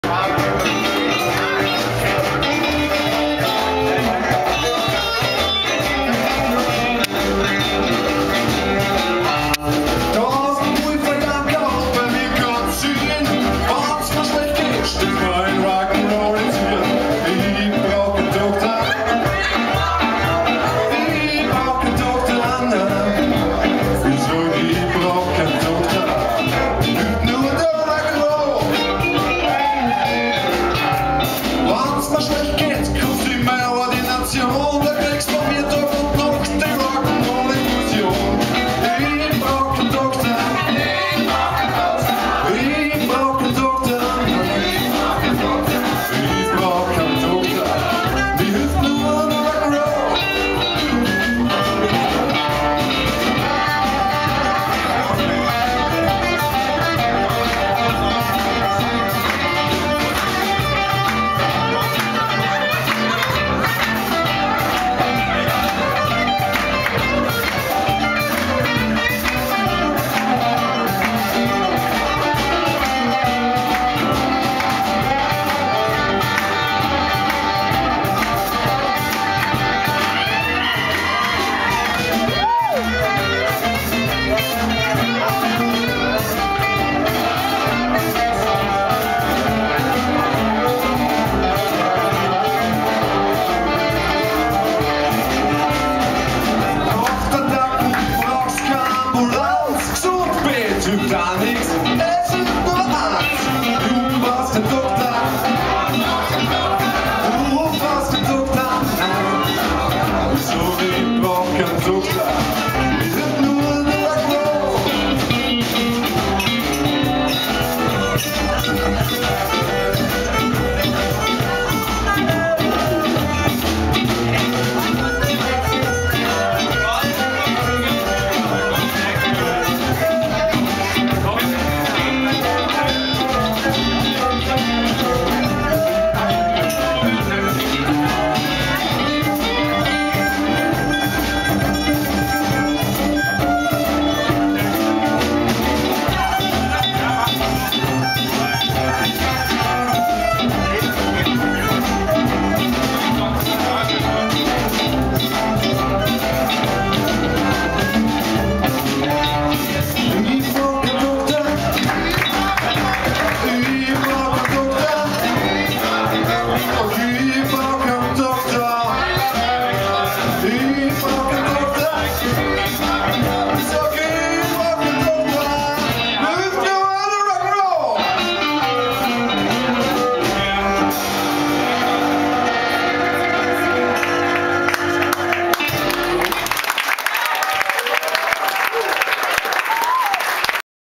Blues Session by Jörg Danielsen mit Börns FunkyFingers III
Bluessession by Jörg Danielsen
mit Bernhard Fellner "Börns FunkyFingers", Stefan Stöhr, Mathias Klos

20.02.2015 - Bierkanzlei, Manfred Hofmann, Vienna
(Blues, Rock N Roll)

vid: websnweb